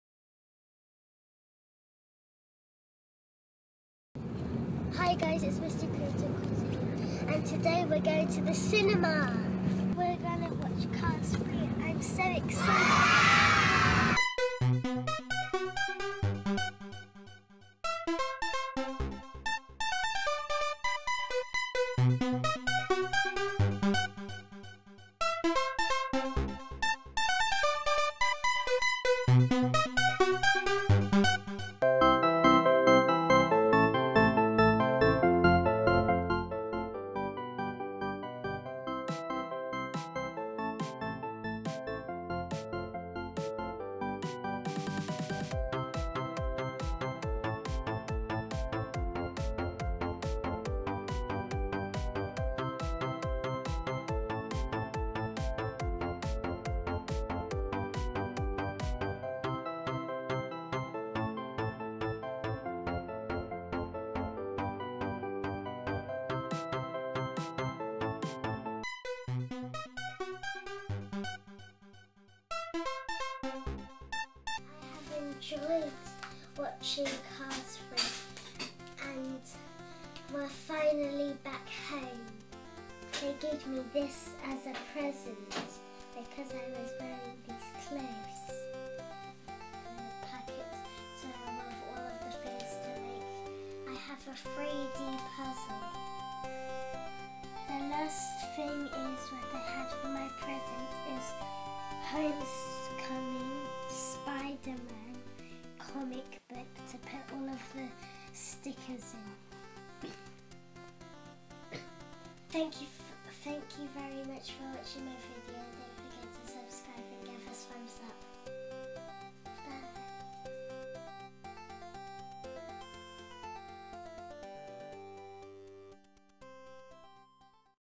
Cinema Trip || Cars 3 Movie || mrcreativequarter
It is summer time so we went to cinema to watch Cars3 Movie! We totally enjoyed this film from the very start to its end. This activity made little one's happy and keep away from boredom!

Hoping that you will watch Vlog in full view!

Cars 3 Costume
Camera used Canon EOS
Battery charger